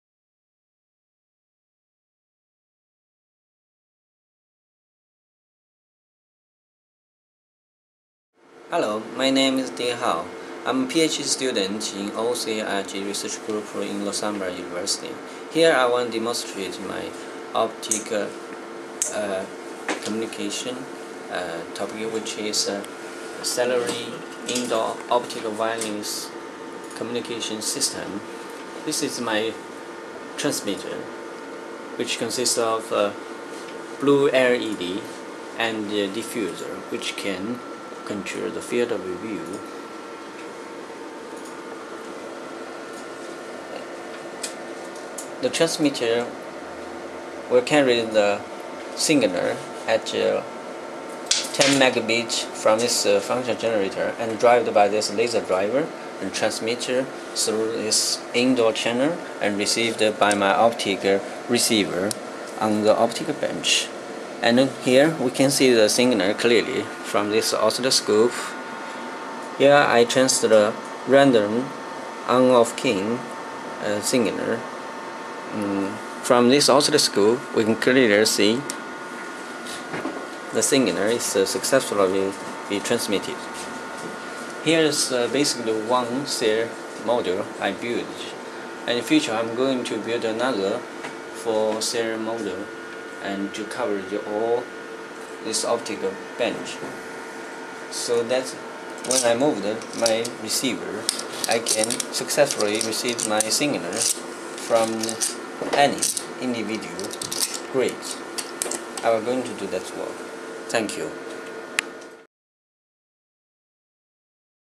Cellular indoor Visible Light Communications (VLC) -- Optical Wireless Communications (OWC)
Presenter: Mr. Dehao Wu -- OCRG, CEIS, PhD Student

In this video, we present a practical indoor diffused cellular visible light communication (VLC) system. It operates at a date rate of 5 Mb/s using the on-off keying non-return-to-zero (OOK-NRZ) modulation format. Using commercially available Luminit holographic light shaping diffusers LSD, the distributions of received power and the bit error rate (BER) are more uniform. The range and coverage area of the cellular link are therefore significantly extended.